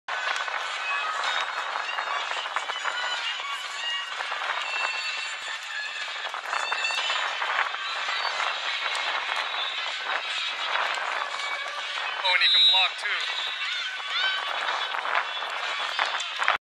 Picking Up The Blitz
Pause tape at impact and see how the blocker drops down and extends through the blitzing player. Gives him a really good rap and lets him know what's gonna happen every time he comes through. As you can see the message was received. Whipping the other guy is how you win football games, and nobody whips guys like Ryan Jordan Whips guys!